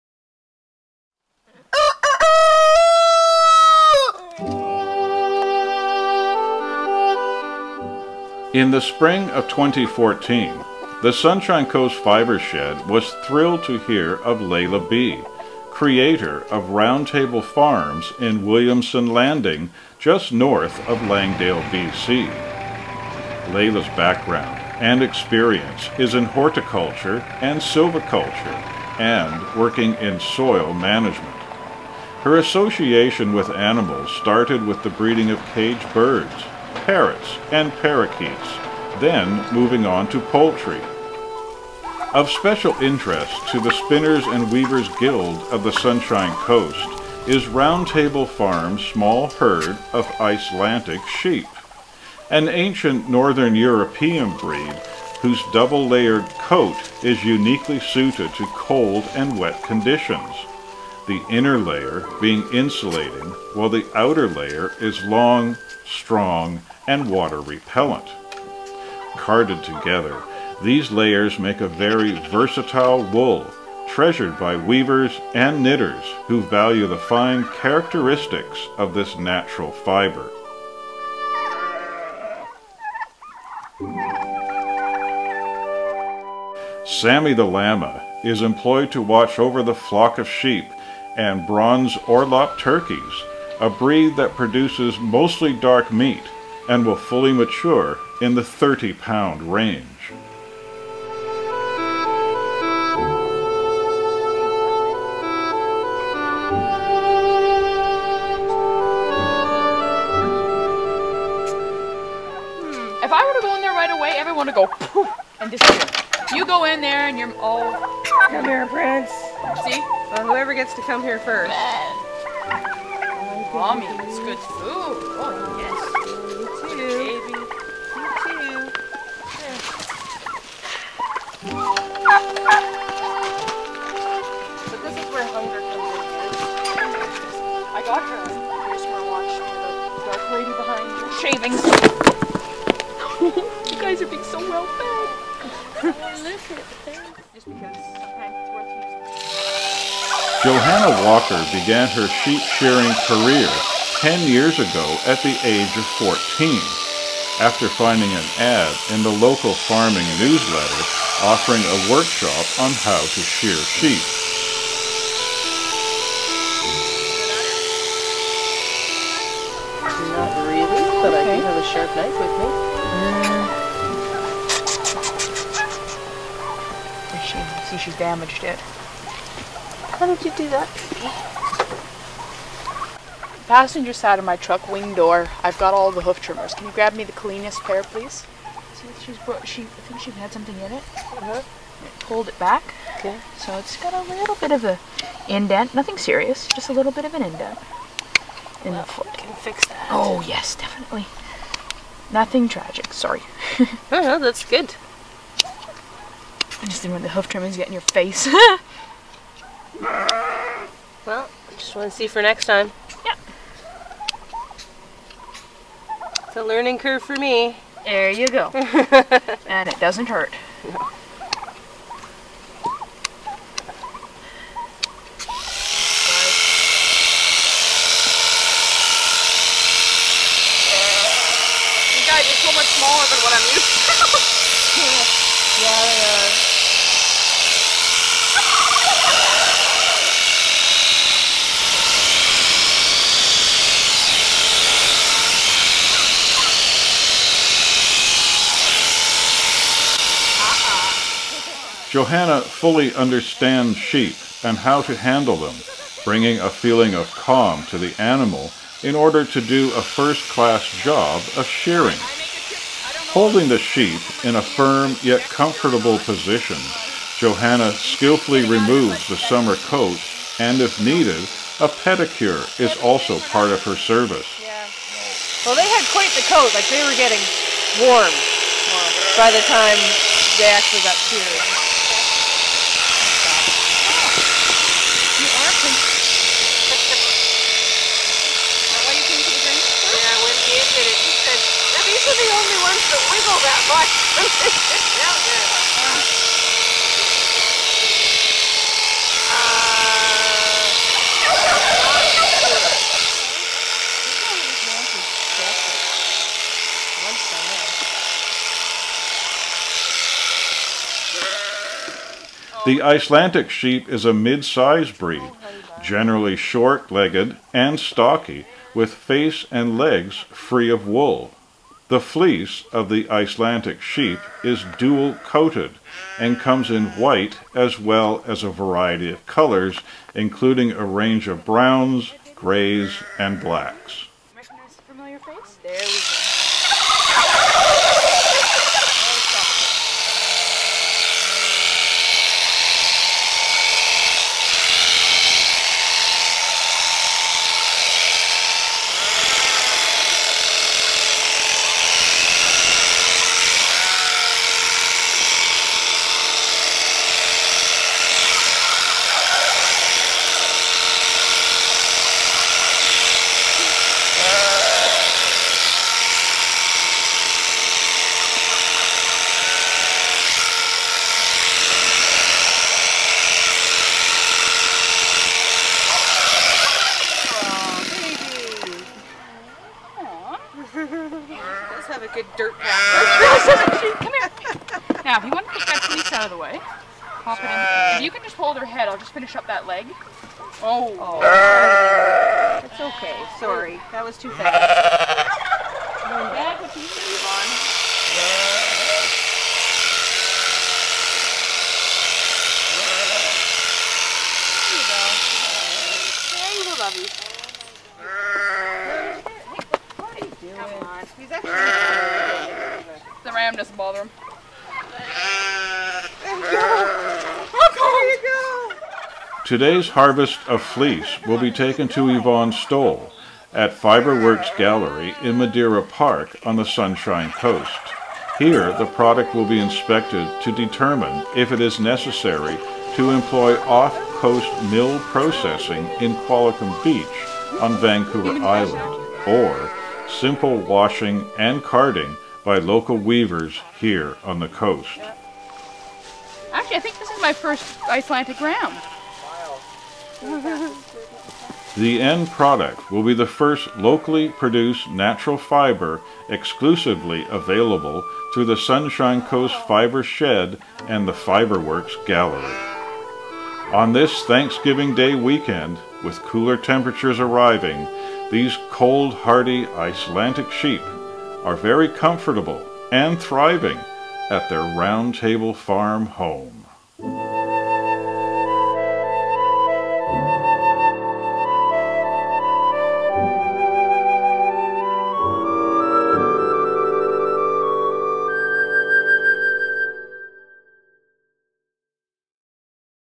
Round Table Farms
Icelandic Sheep Shearing from Round Table Farms in
Williamson Landing, Sunshine Coast BC